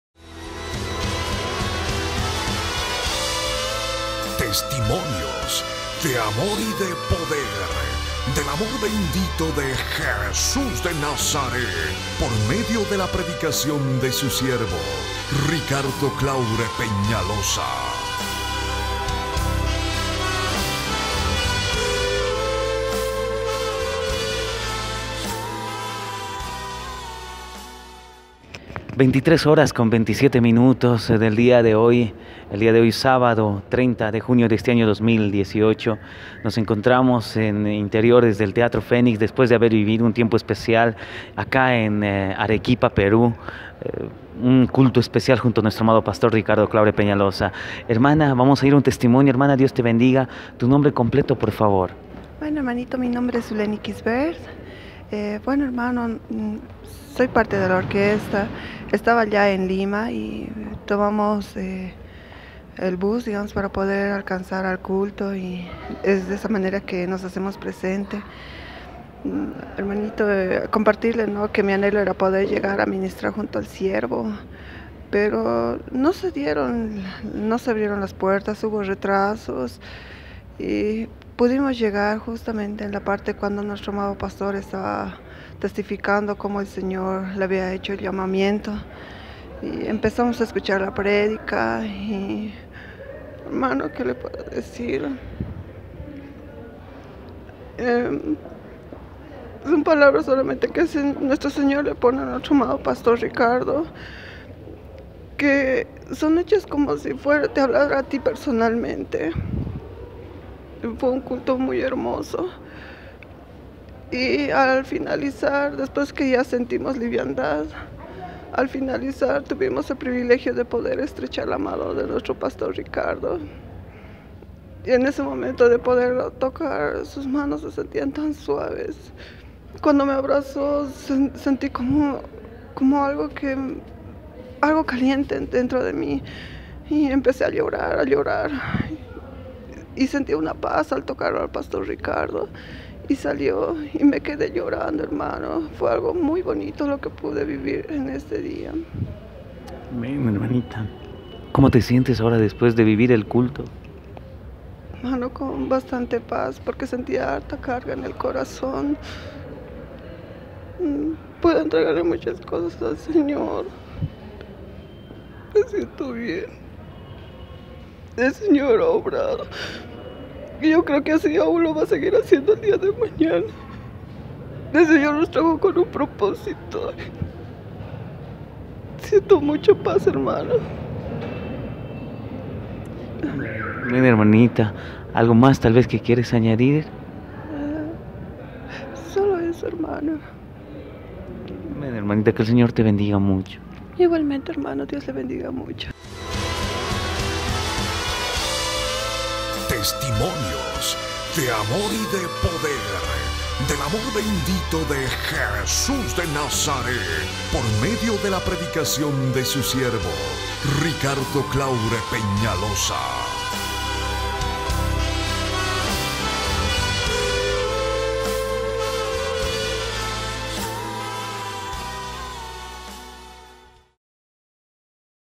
RICARDO CLAURE PEÑALOZA (PASTOR):TESTIMONIO DE AMOR Y DE PODER -Zuleny/Perú, 2018
Arequipa, Perú
Zuleny Quisbert
Sábado, 30 de junio de 2018
"... Son Palabras que Nuestro Señor Jesucristo le pone a nuestro Pastor Ricardo... Fue un culto muy hermoso..."

Acerca de la Palabra llena de Amor y Ternura que oyó por medio de la predicación de nuestro amado Pastor Ricardo Claure Peñaloza, Vocero esforzado de Dios, en la ciudad de Arequipa, Perú (Teatro Fénix).